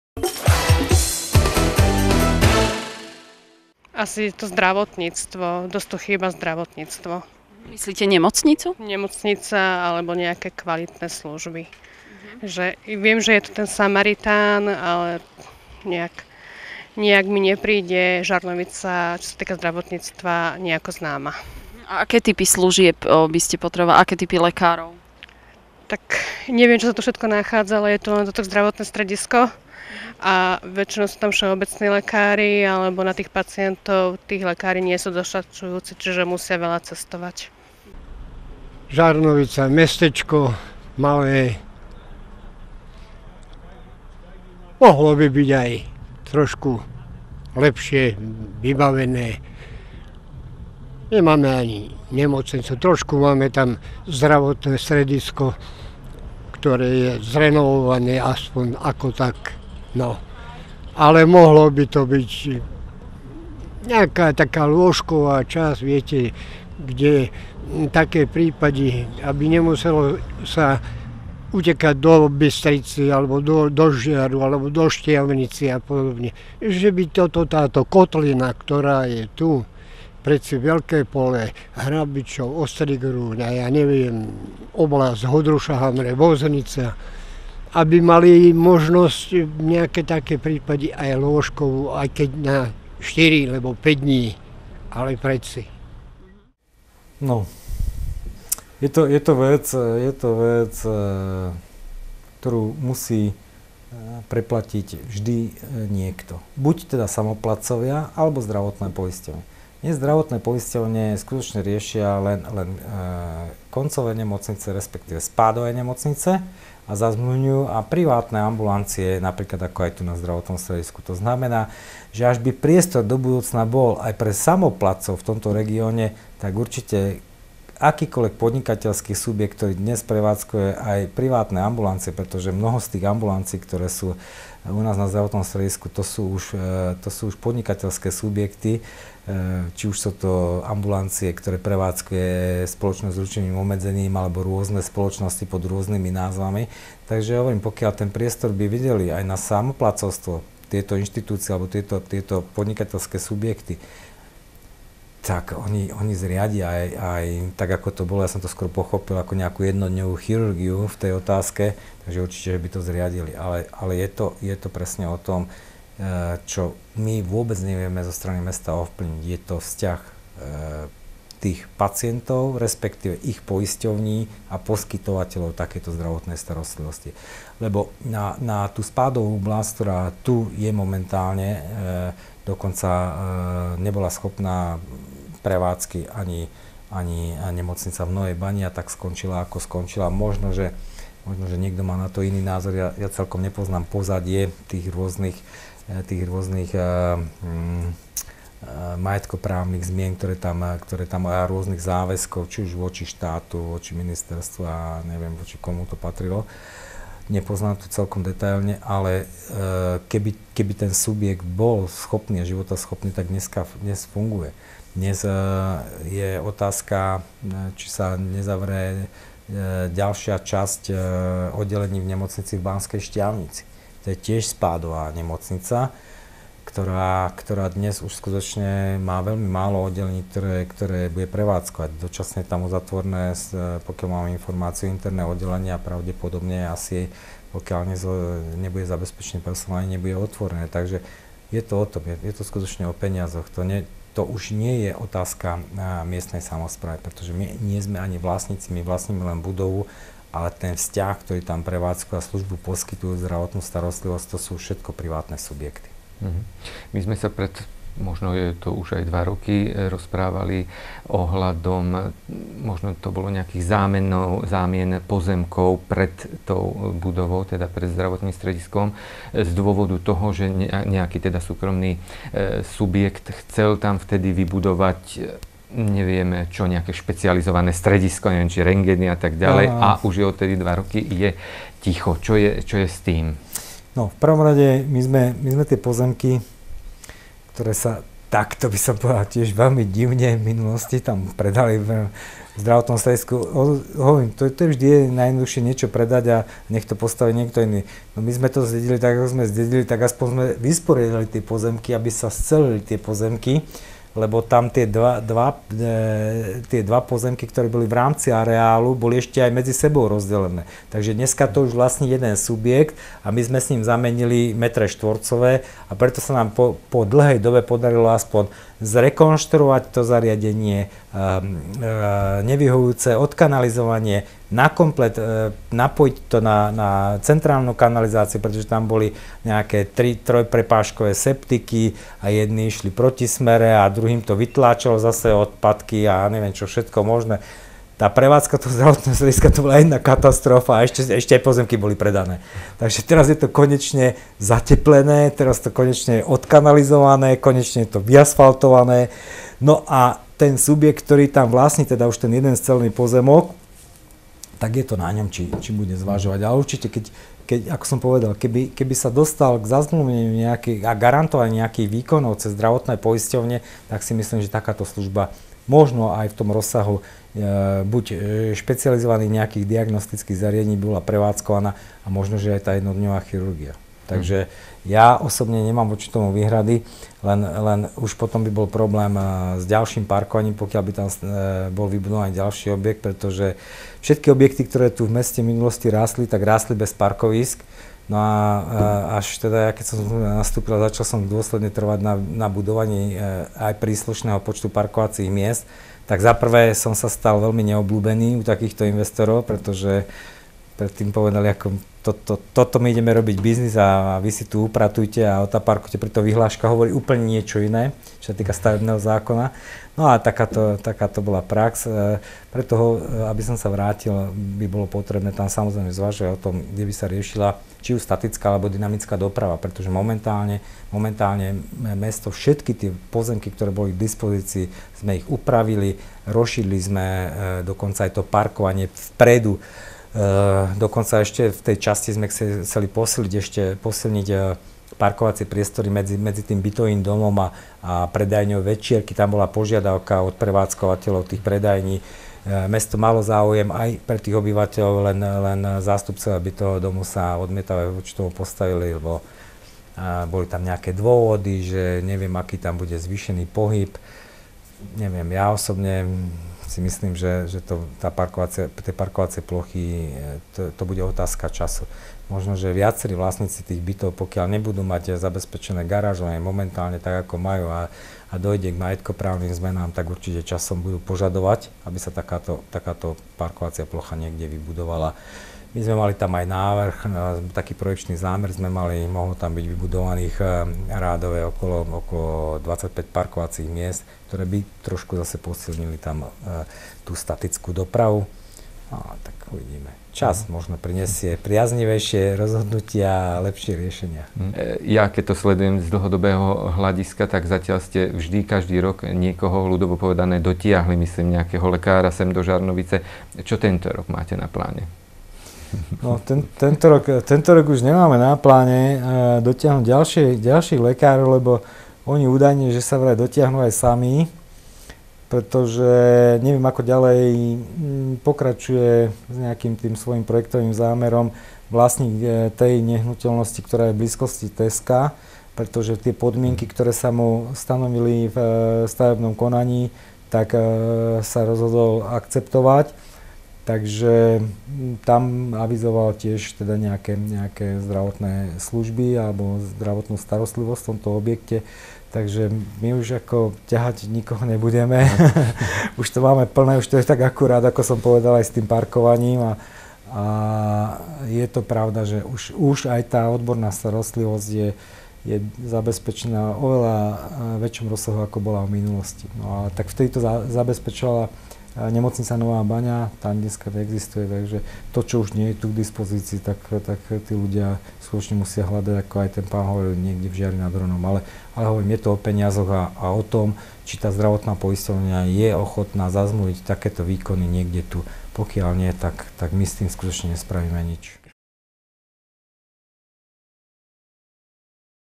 Jednodňová chirurgia v Žarnovici
Kamil Danko o zdravotnej službe v Žarnovici.